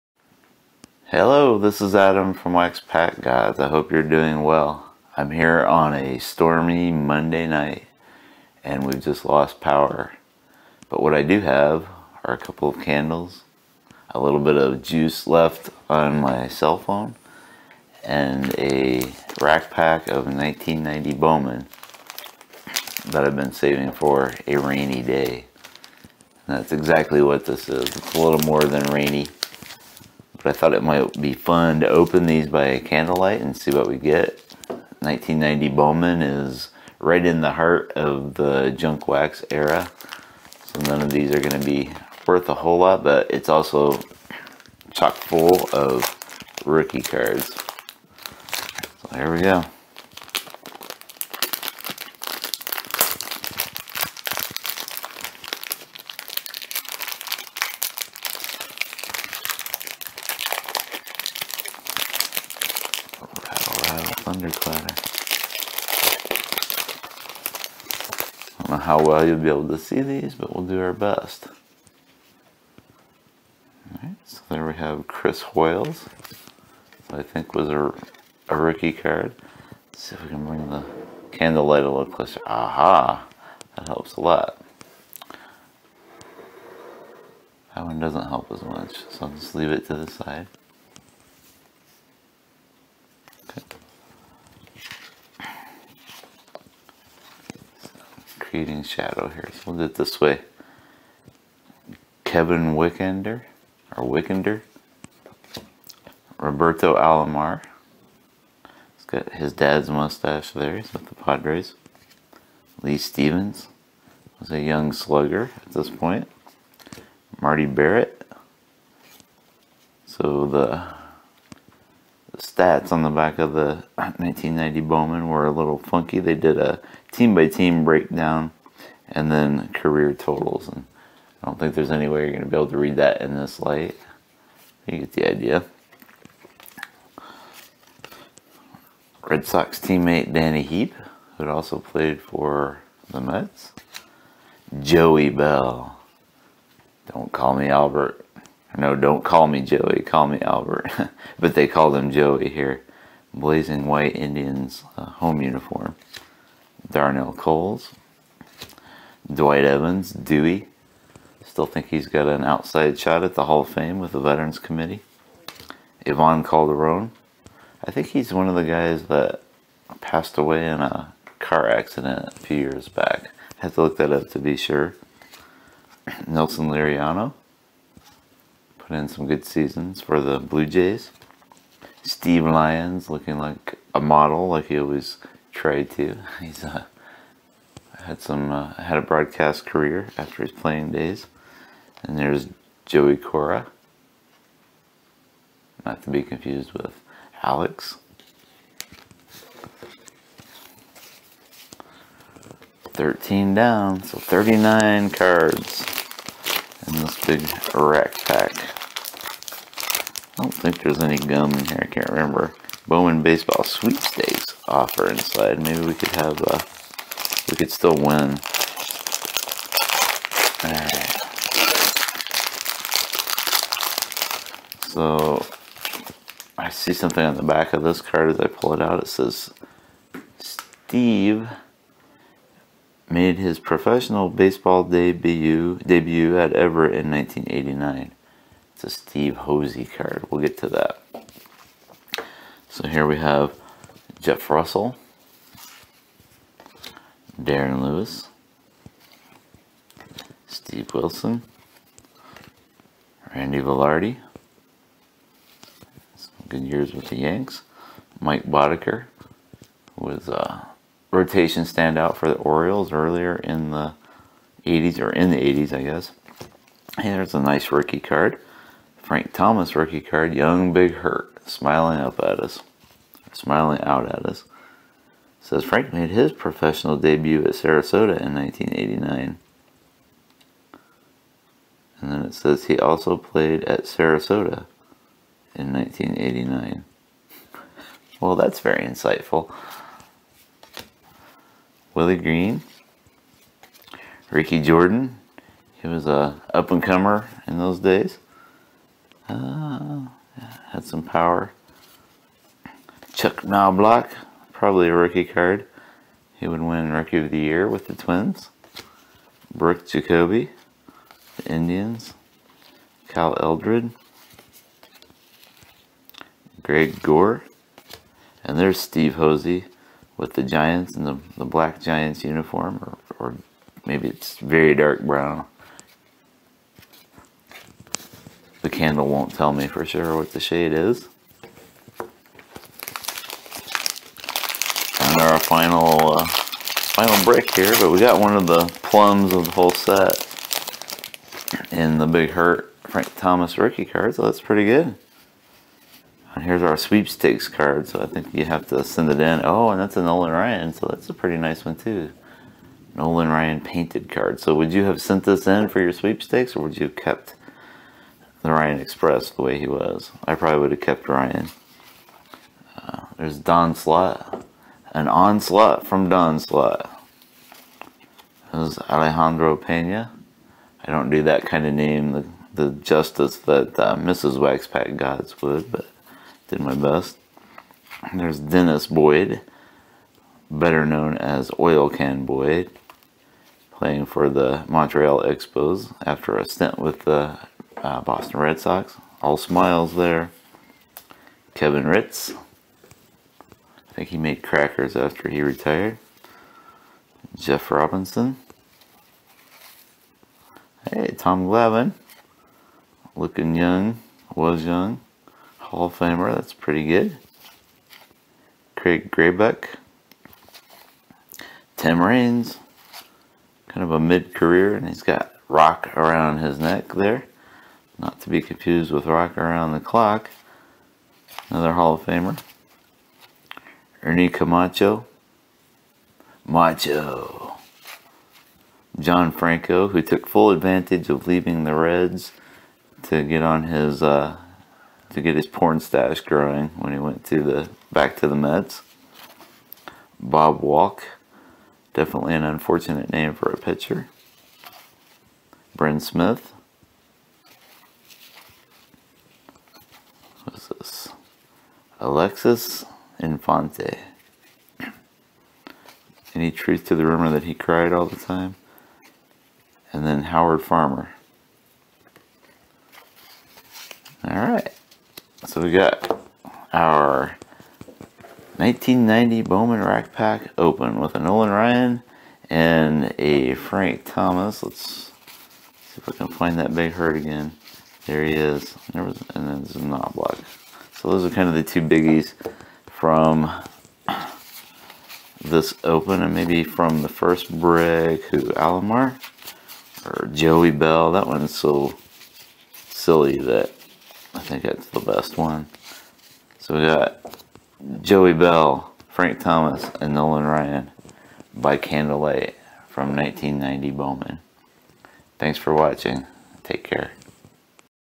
1990 Bowman Break by Candlelight - Frank Thomas, Nolan Ryan, and Thunderstorms
1990 Bowman Break by Candlelight - Frank Thomas, Nolan Ryan, and Thunderstorms
We were without power for about 30 hours this week, but I *did* have a little juice left in an old cell phone, a couple of candles, and a 1990 Bowman rack pack I'd been saving for a rainy day.

This one fit the bill.

Tune in to see who I pulled!